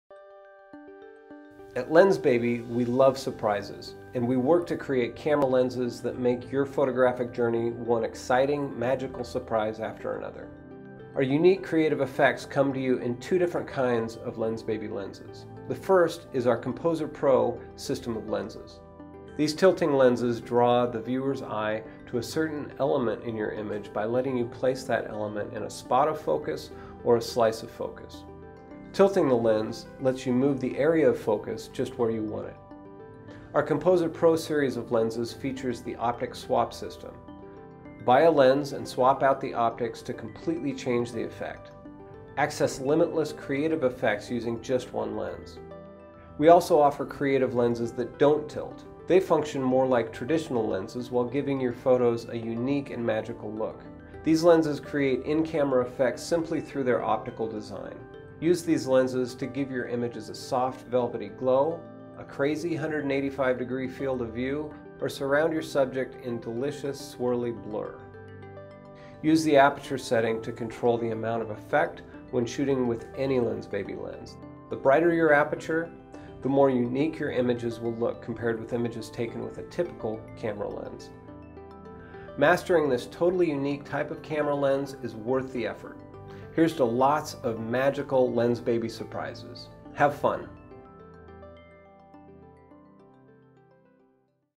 What is Lensbaby
Learn about Lensbaby's creative effects camera lens system from Chief Creative Officer & Co-Founder Craig Strong